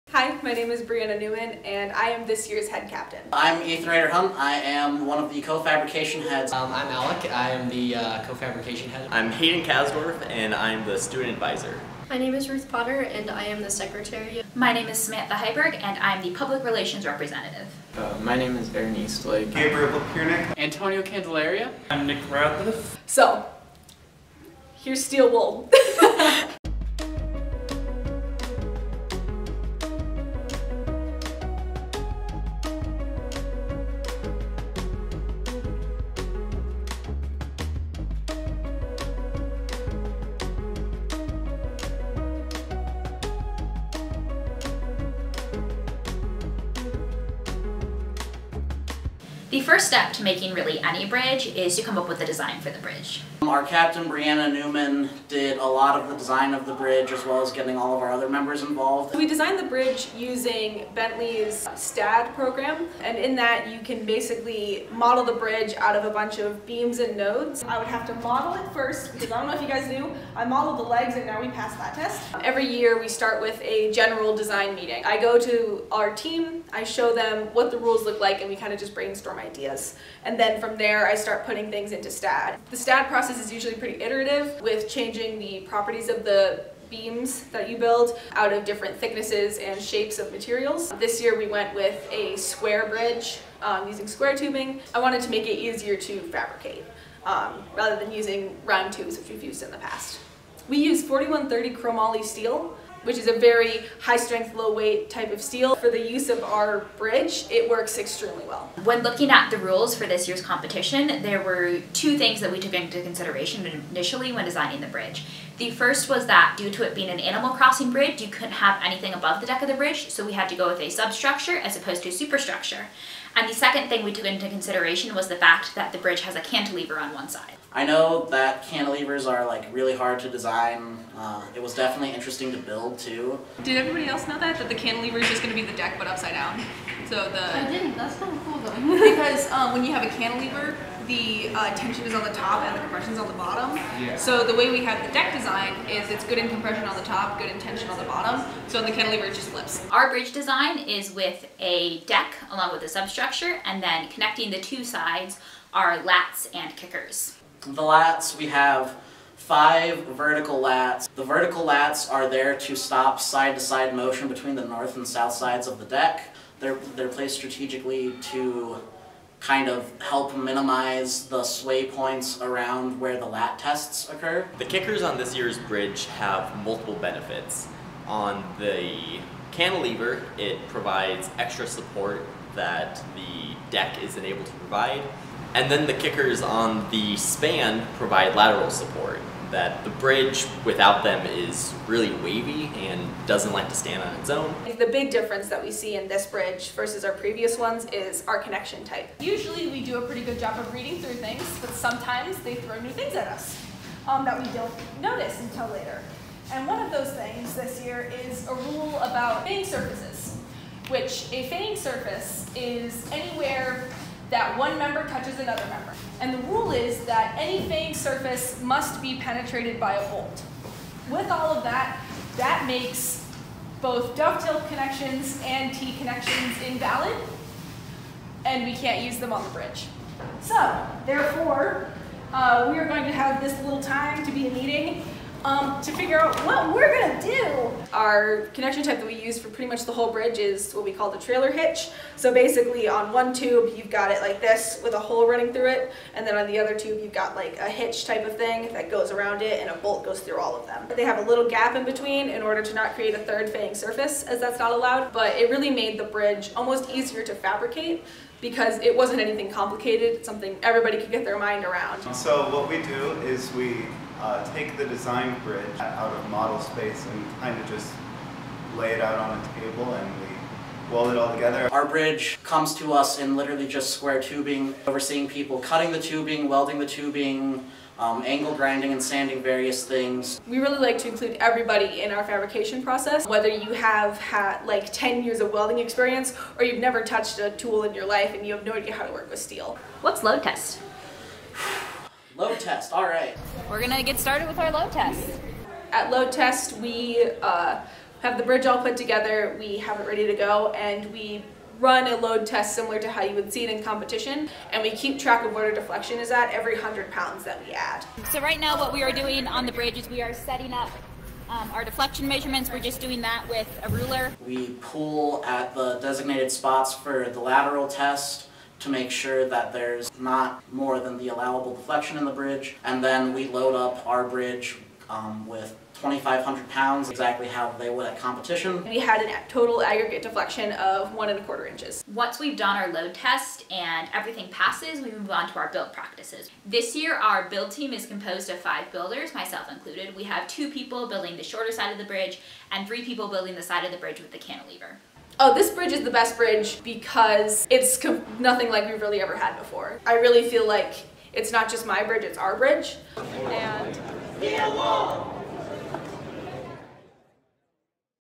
South Dakota Mines 2022 Steel Bridge Video
Here is our submission for the 2022 AISC SSBC optional video competition.

This Years Team!
Captain: Brianna Newman
Fabrication Heads: Ethan Rader-Hunt and Alec Colgan
Student Advisor: Hayden Casdorph
Treasurer: Alec Colgan
Secretary: Ruth Potter
Public Relations: Samantha Heiberg
General Members: Aaron Eastlake, Antonio Candelaria, Gabriel Papiernik, Nick Radliff, Carter Amland, Alex Hall, Duncan Pilling, Caden Caylor, Sophia Sorbo

Video Filmed and Edited By Samantha Heiberg